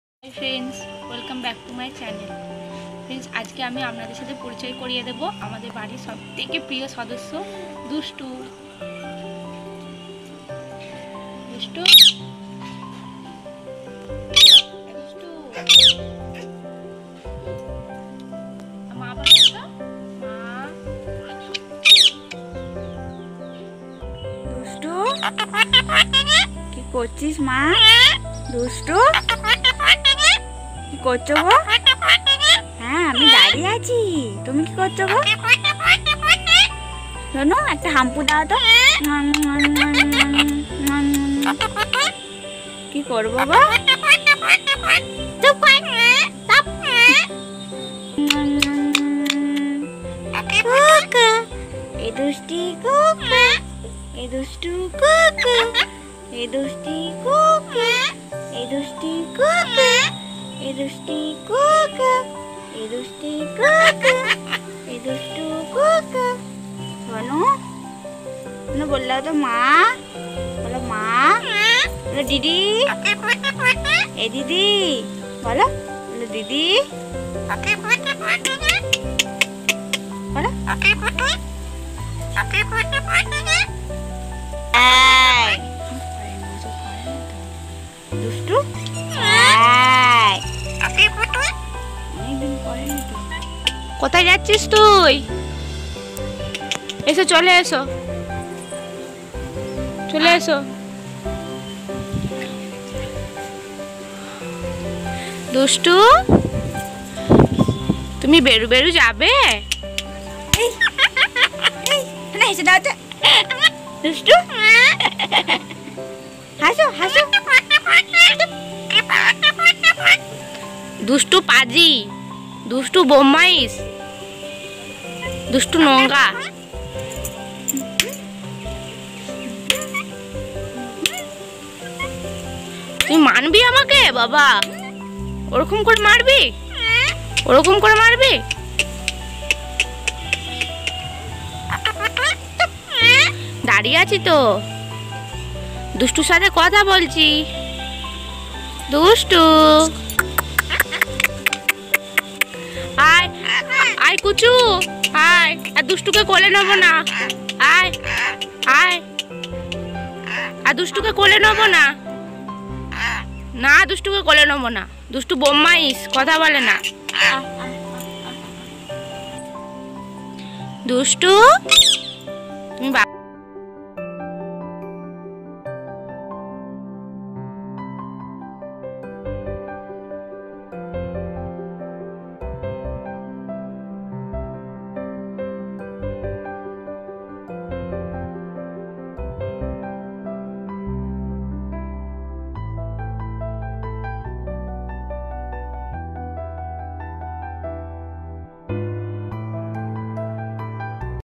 Vlog with Parrot / Dustur sange kichu muhurto / Bengali Vlog
Hi friends, This my vlog with parrot. I hope you watch this video.